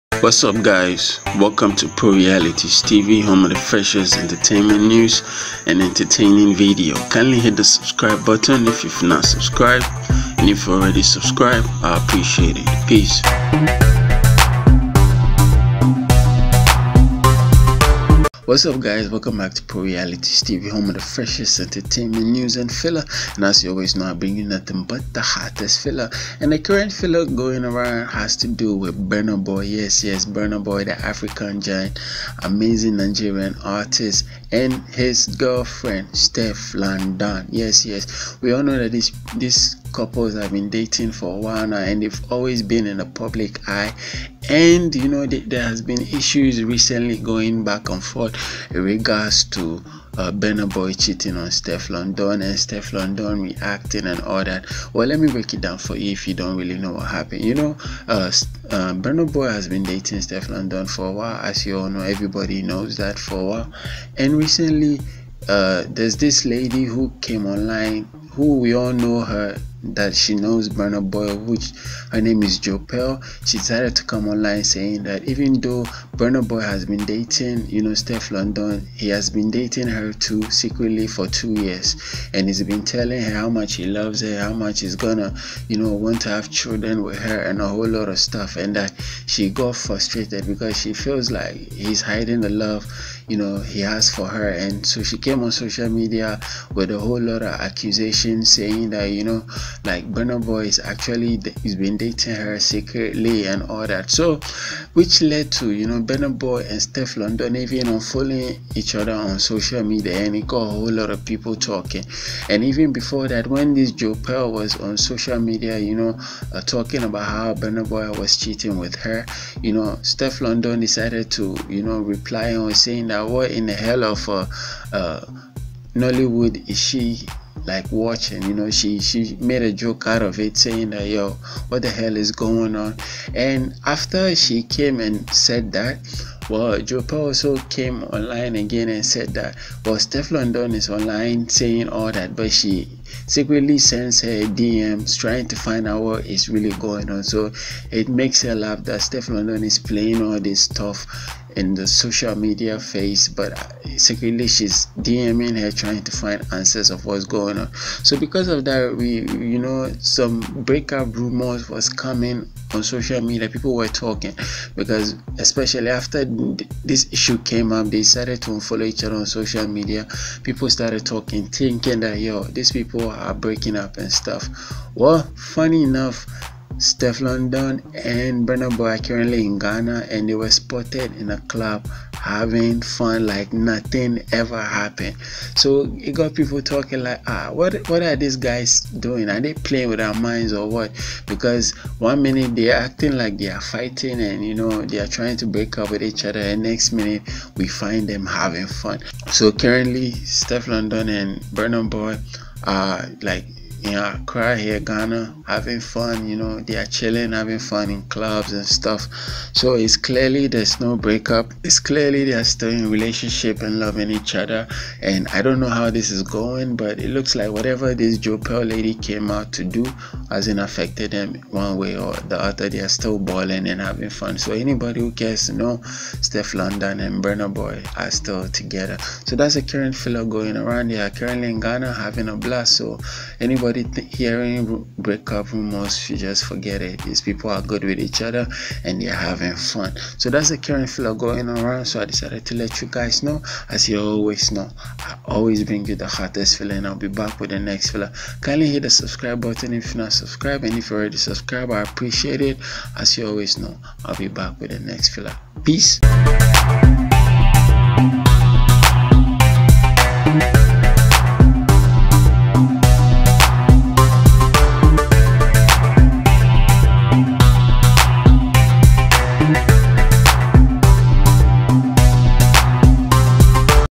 BURNA BOY AND STEFFLON DON CHILLING IN GHANA AFTER BREAK UP RUMOURS
NOTE - UNAUTHORIZED UPLOAD OF THIS CONTENT IS PROHIBITED.